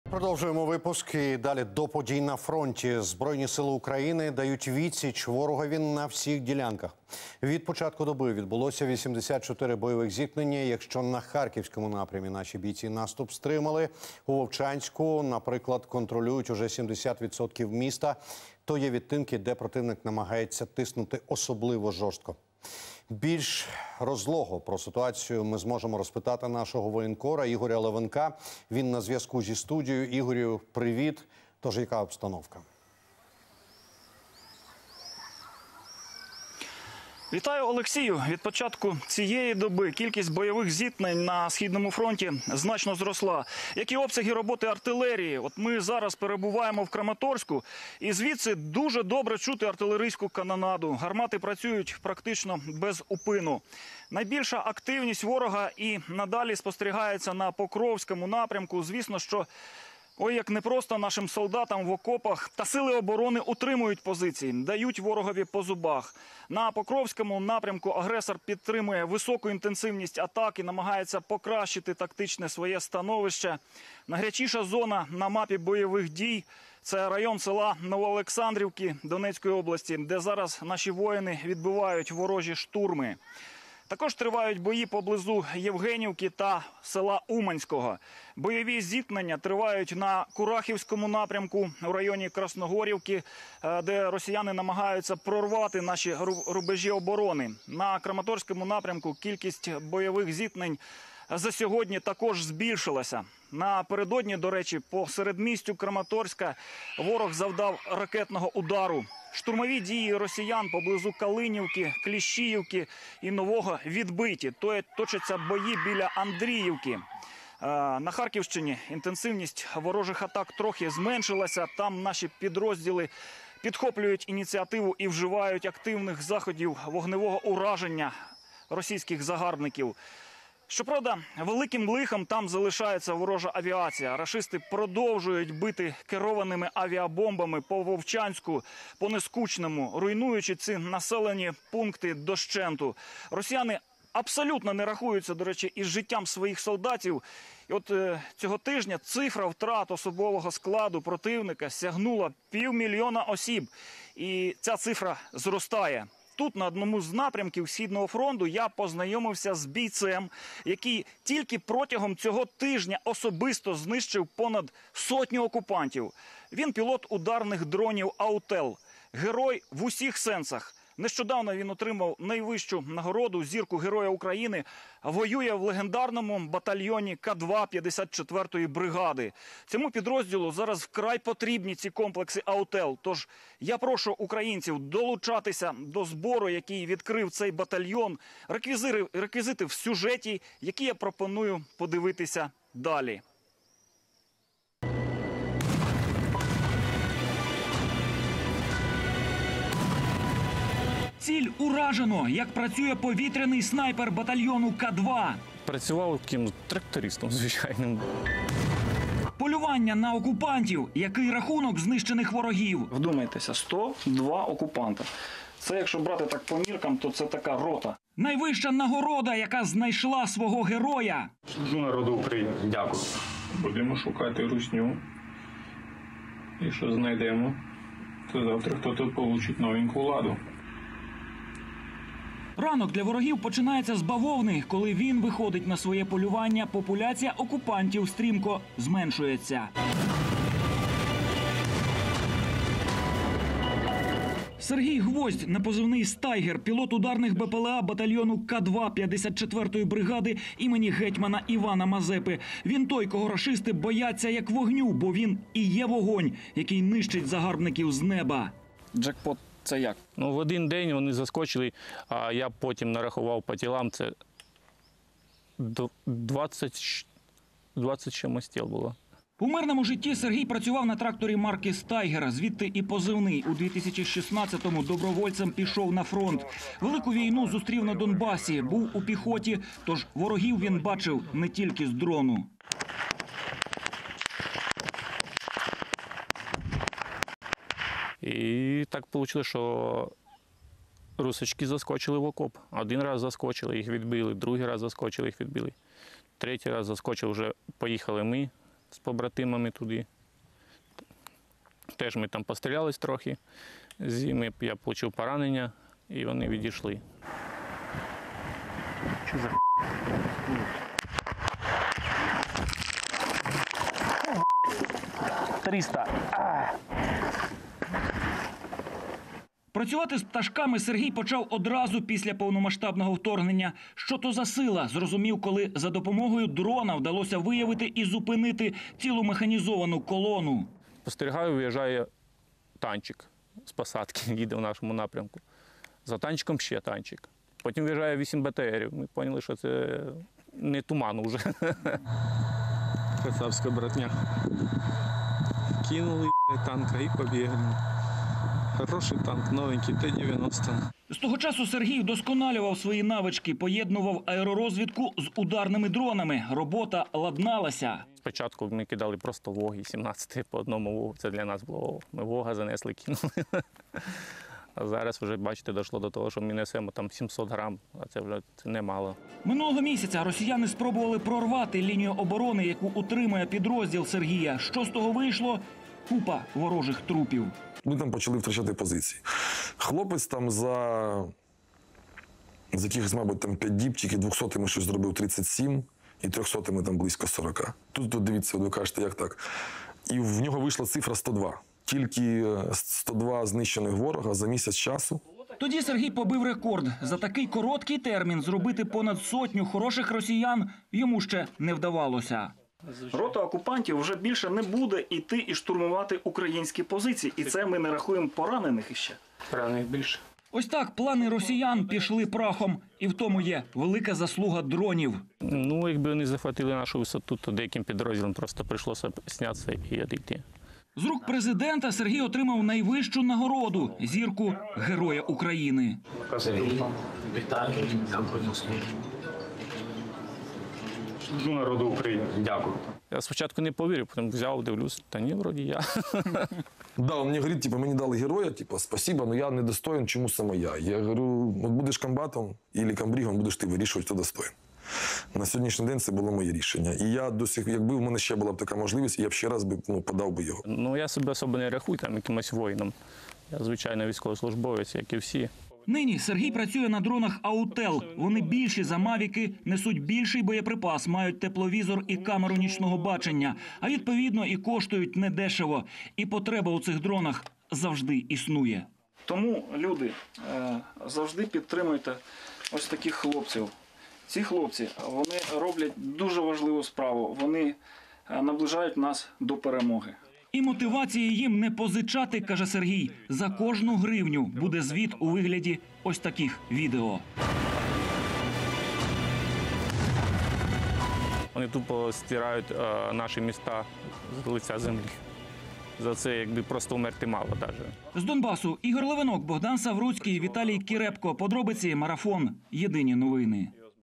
ВМЕРЛИ ЗА ЛАДУ! Сотня "двохсотих" росіян за тиждень! Як ударні дрони знищують окупантів?
Юний пілот ударних дронів "Autel" 54-ї бригади, Герой України, Сергій Гвоздь знешкоджує десятки окупантів. Маючи досвід в піхоті ще з Донецька, боєць вміло прораховує окупантів у бою? Який військовий досвід має Сергій та яка специфіка роботи його дронів? Подробиці розкажемо в сюжеті.